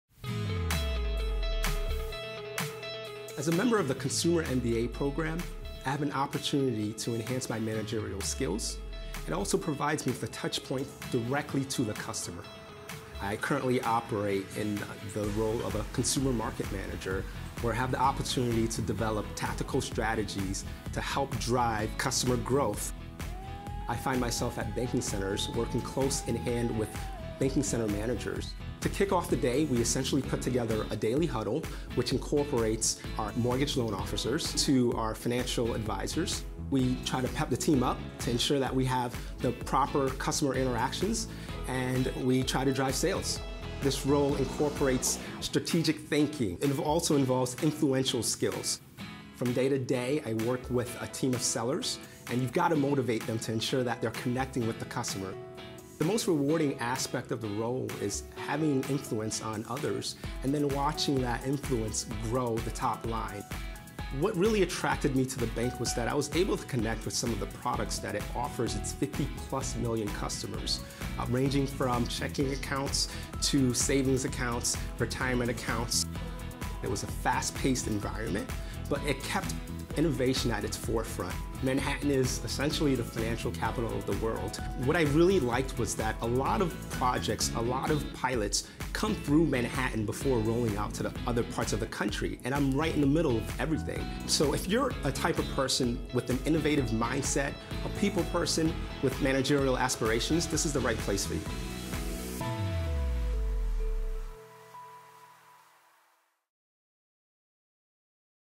Bank of America Consumer MBA Associate
Jerry Decembre talks about his experience in the Consumer MBA Program with Bank of America.